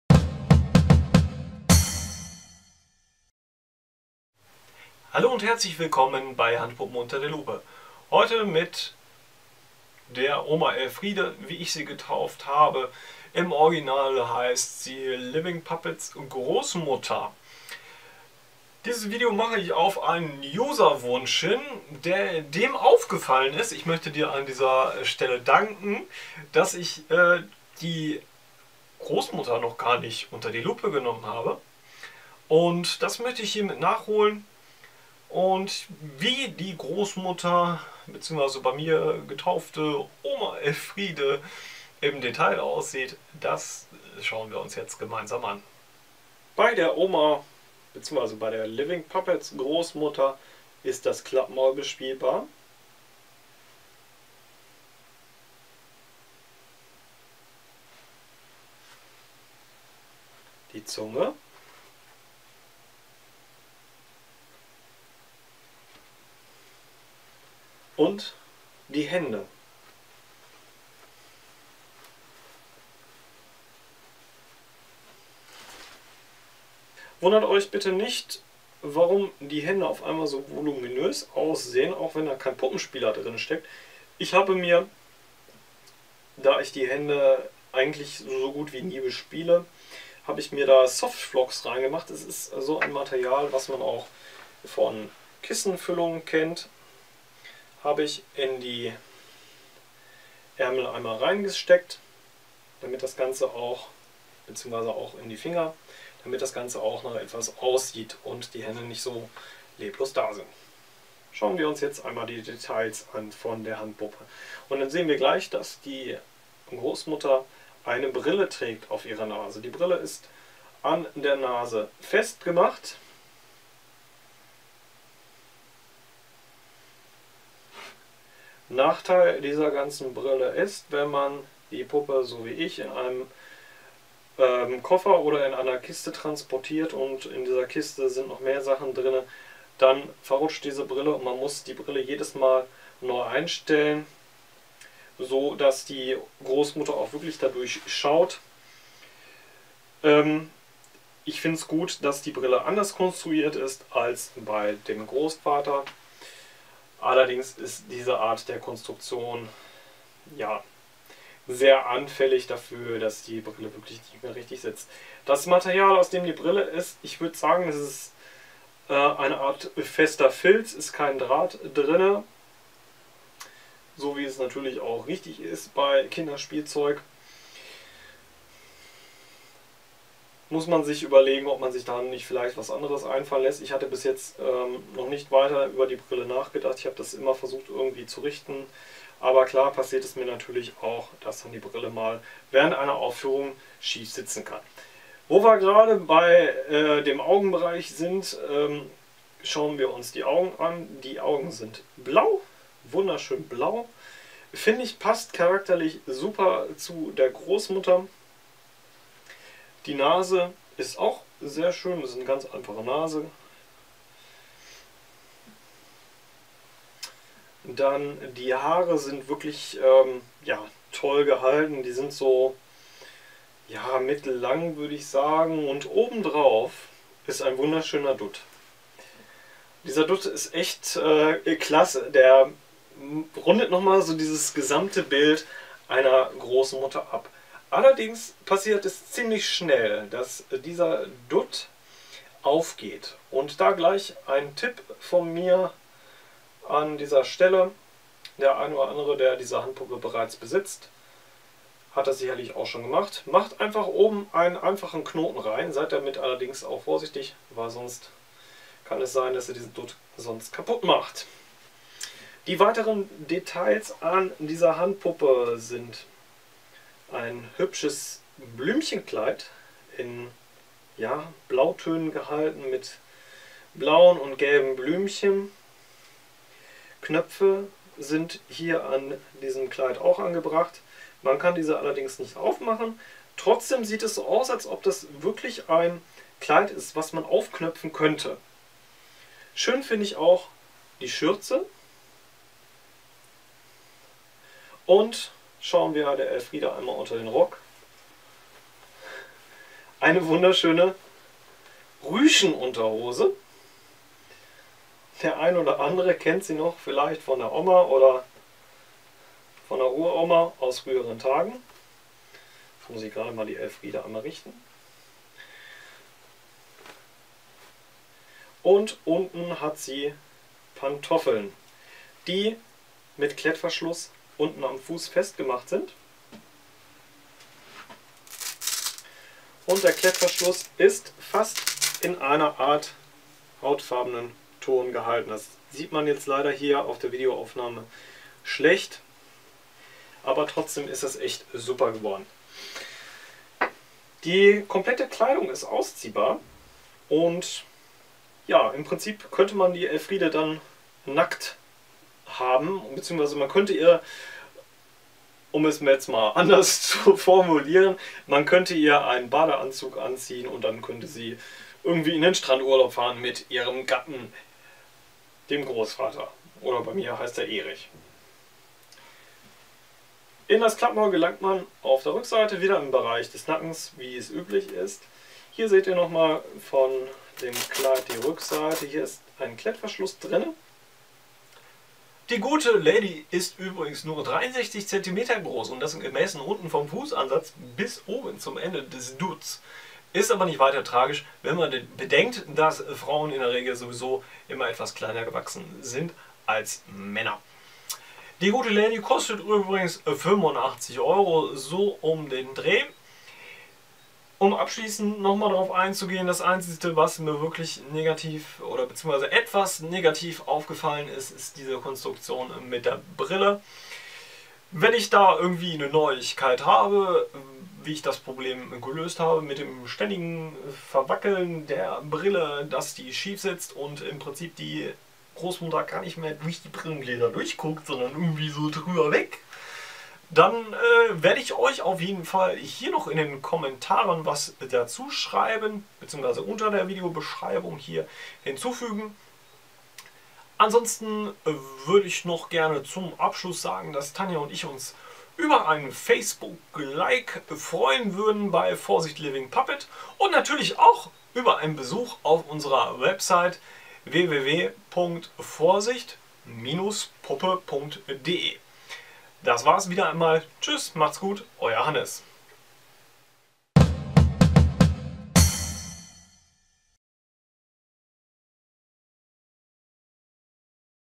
Handpuppen unter der Lupe - Großmutter von Living Puppets
VIDEOBESCHREIBUNG

Die Großmutter von Living Puppets will noch mit unter die Lupe genommen werden. Sie ist sehr gut gearbeitet und was noch so besonders an ihr ist, dass erfahrt ihr in unserem "Handpuppen unter der Lupe"

PRODUKTDETAILS

Produktname: Großmutter
Hersteller: Living Puppets/ Matthies Spielprodukte
Preis: ca. 92 Euro
Größe: ca. 65 cm, nachgemessen: 63 cm bis zum Dutt

Danke für deinen Besuch!